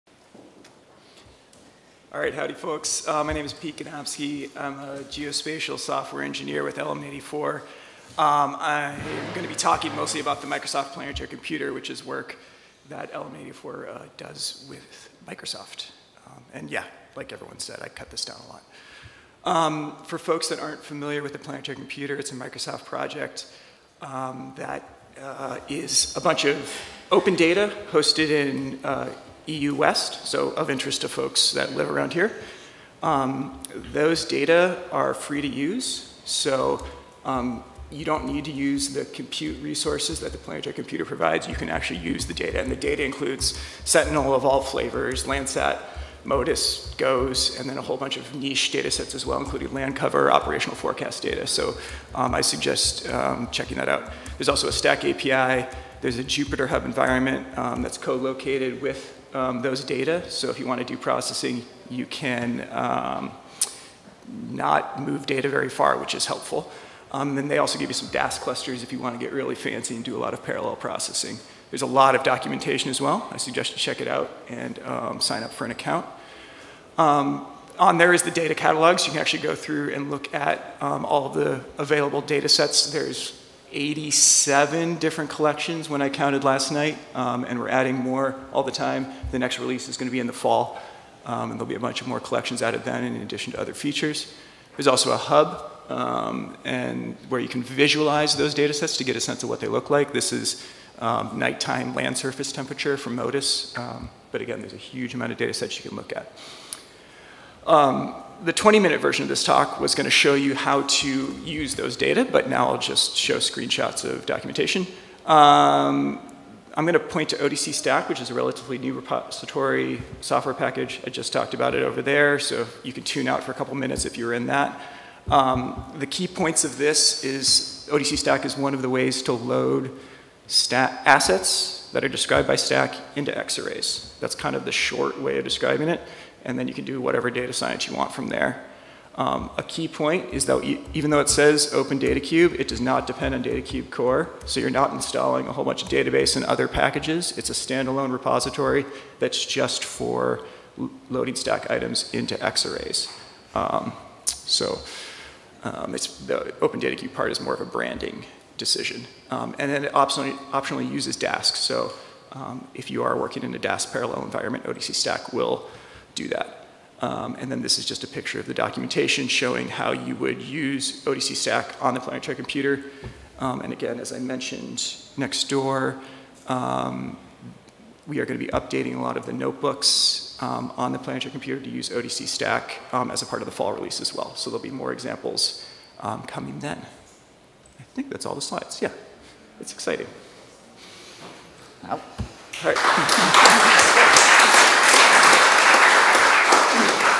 FOSS4G 2022 | Exploring Data Interoperability with STAC and the Microsoft Planetary Computer
As a part of its AI4Earth initiative, Microsoft has created a Planetary Computer (PC) for hosting and processing open geospatial data. In addition to publishing a wide range of datasets, including Sentinel-2, MODIS, and more, the PC provides a powerful API and compute system based on open-source geospatial tools and using STAC metadata for data query, discovery, and access. In this talk, we present the latest in open geospatial data access, discovery, processing, and visualization using a variety of datasets from the Planetary Computer. We demonstrate use of the odc-stac package, which leverages the power of the OpenDataCube computing platform without the need for a database backend, and how odc-stac can load, mosaic, and transform geospatial assets into xarray datasets. We dive into other data interoperability tasks, including scaling processing with Dask and leveraging a variety of cloud-native formats. Along the way, we provide recommendations for data providers and curators on how to ensure their data can be used in a rich, interoperable way by the latest in geospatial processing tools.

Pete Gadomski